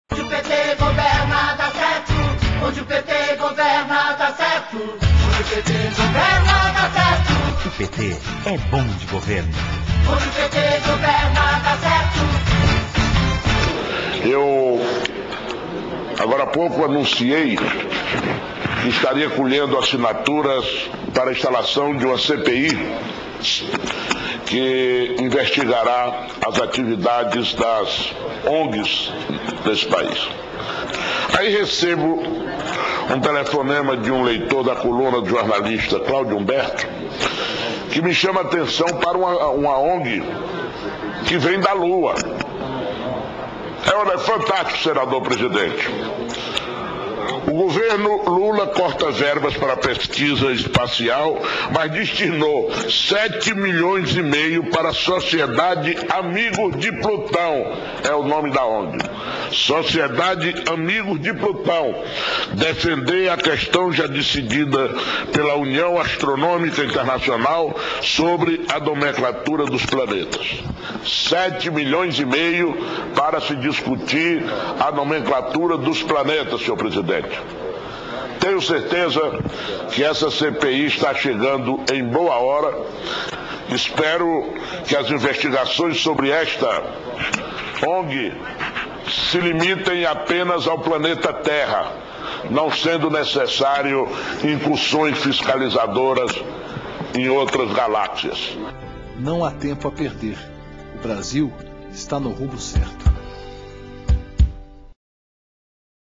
Ong: Amigos de Plutão.
O Senador Heráclito Fortes estava colhendo assinatura para instaurar uma CPI para investigar a farra das ONGS (Organização não governamentais) quando foi surpreendido com uma ONG nada convencional.

Seria a Socidade Amigos de Plutão, onde recebeu R$ 7,5 Milhões. O objetivo da ONG seria discutir a nomenclatura dos já conhecidos planetas. Enquanto isso Lula cortava a verda destinado para estudos espaciais